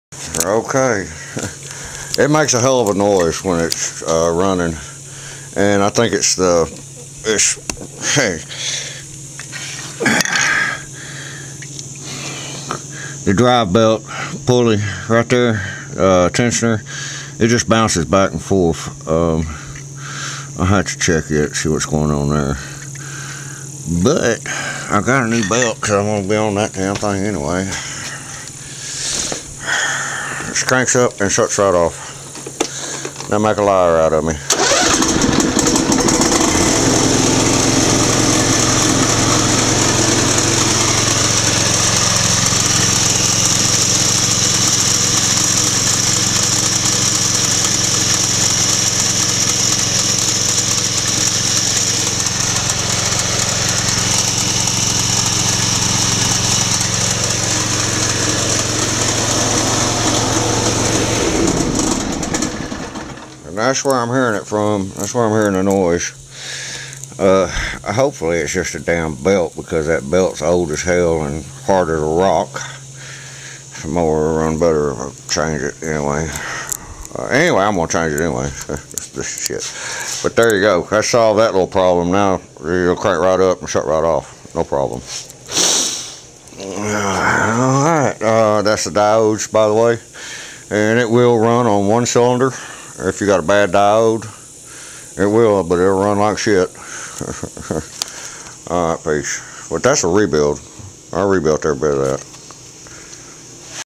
Fires right up shuts right off plus a noisy drive belt tensioner.
26 hp briggs and stratton intek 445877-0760-E1

Peace